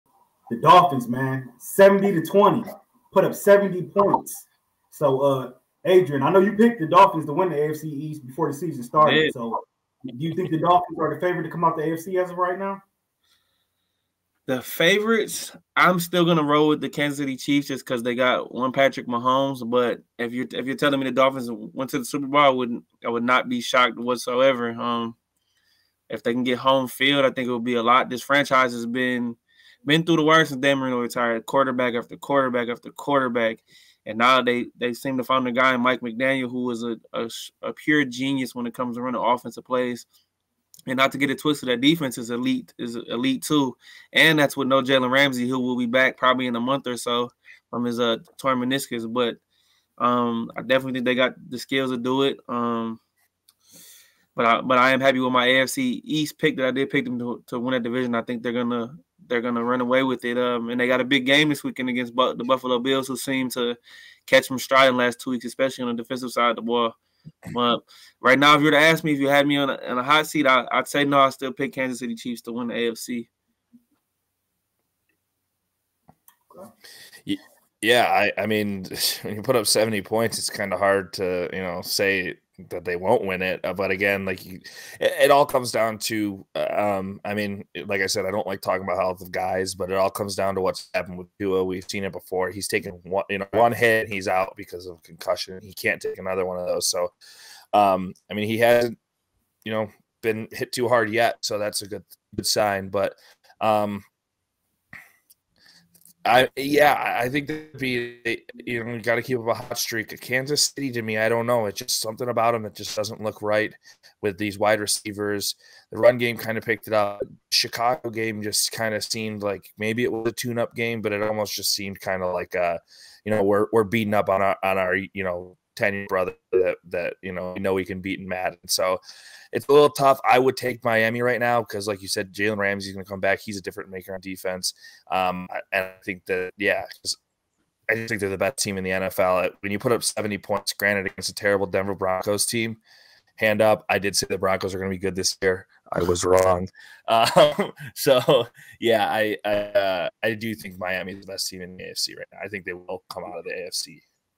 Are The @MiamiDolphins The Team To Beat In The AFC?
Raaj, Adrian & Reid With The Breakdown